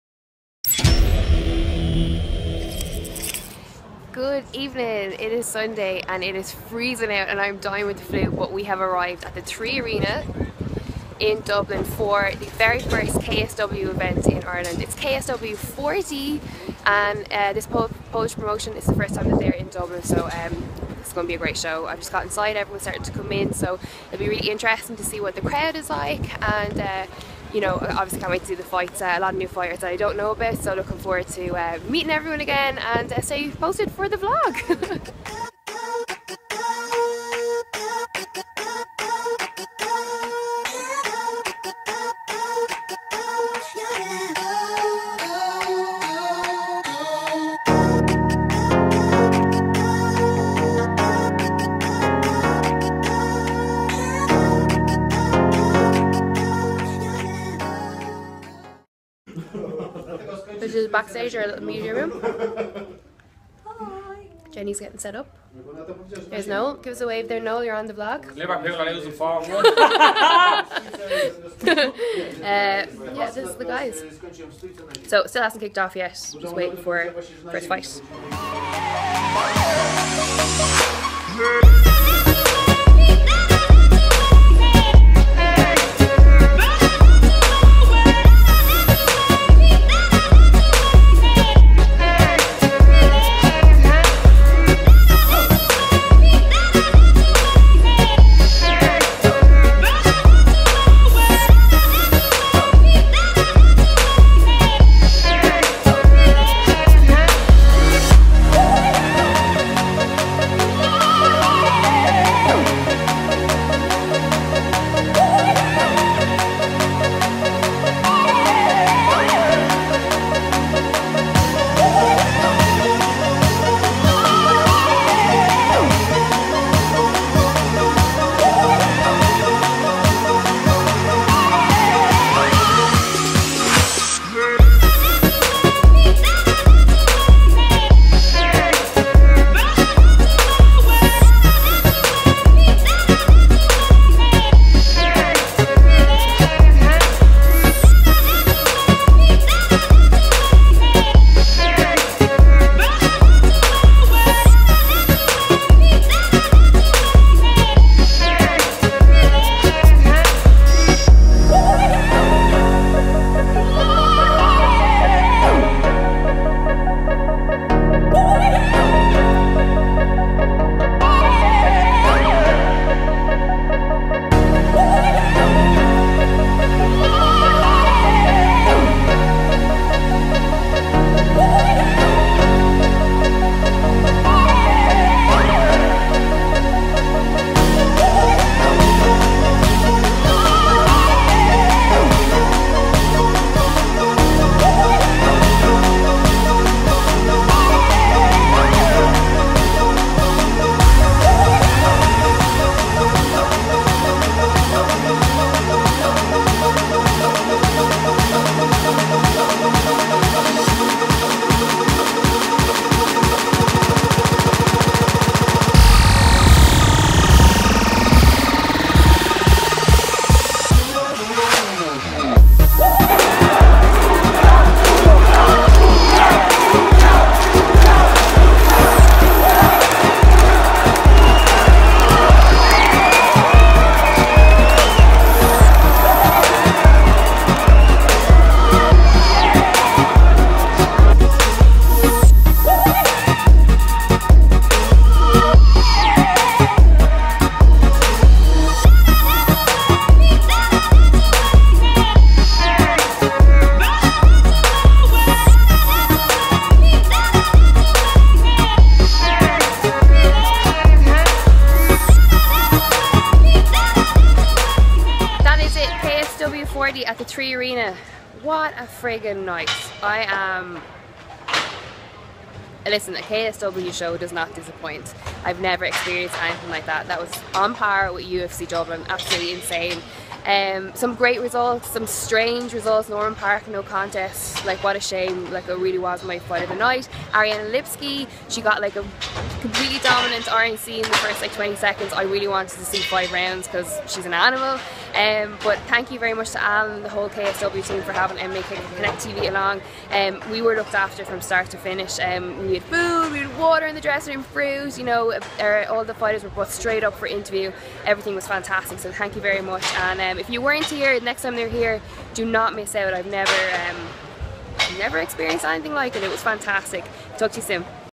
The Vlog : KSW 40 at the 3arena Dublin
Watch back the latest MMA Connect TV Vlog with Lydia Des Dolles from last nights KSW debut Irish show KSW 40 at the 3Arena - a flawless production like nothing we've experienced before, professionally run with a vibrant capacity crowd in attendance!